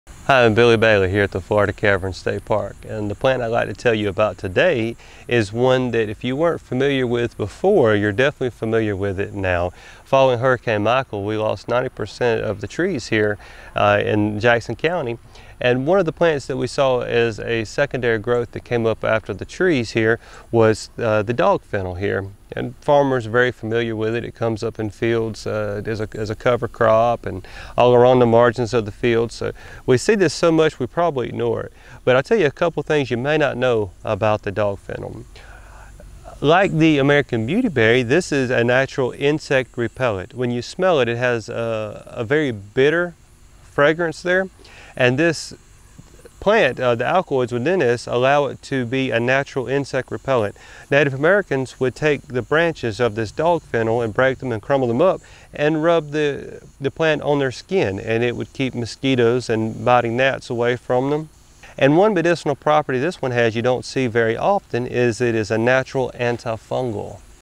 Dog Fennel: Nature at Florida Caverns
Dog Fennel (or Dogfennel) is everywhere in NW Florida since Hurricane Michael. Did you know that insects flee it and that Native Americans used it for that reason? Learn more from Billy Bailey of Florida Caverns State Park: